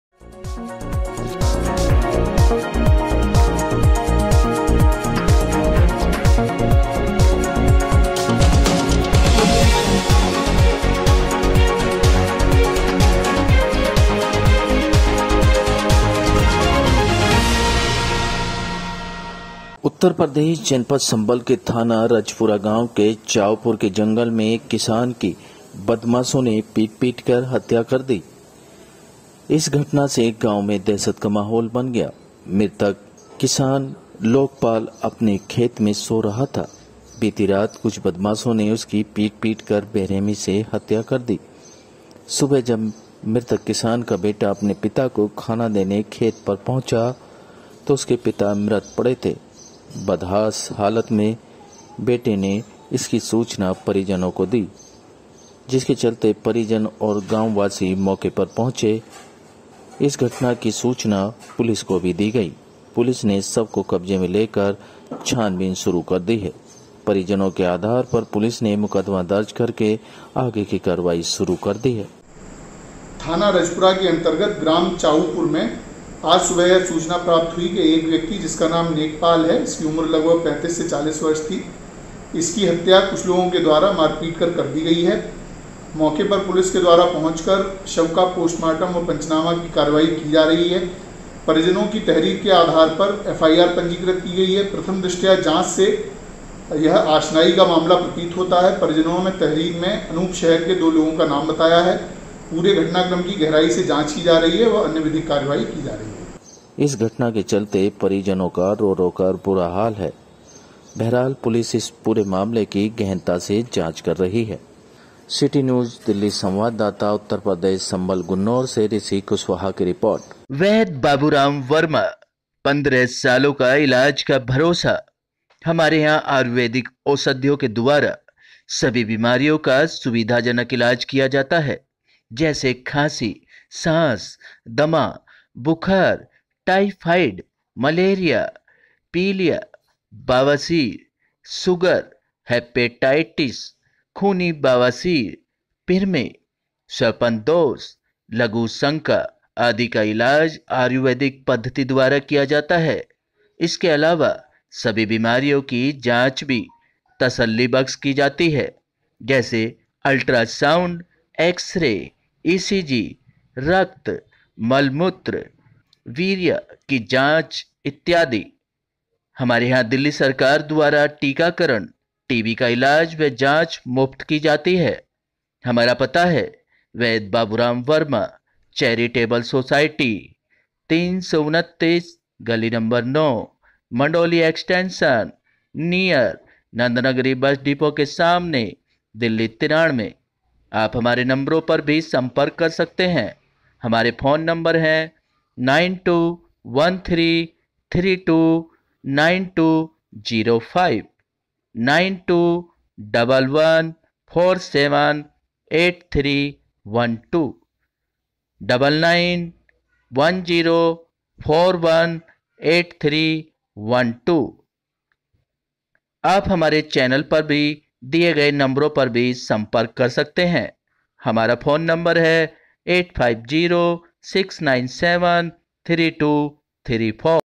CITY NEWS DELHI -बदमाशों ने किसान की पीट-पीटकर की हत्या ।
बदमाशों ने किसान की पीट-पीटकर की हत्या । उत्तर प्रदेश जनपद संभल इलाके का मामला । पुलिस मामले की जांच में जुटी । परिजनों का रो रो कर बुरा हाल । गांव में दहशत का माहौल बना । यूपी में बदमाशों के हौसले बुलंद ।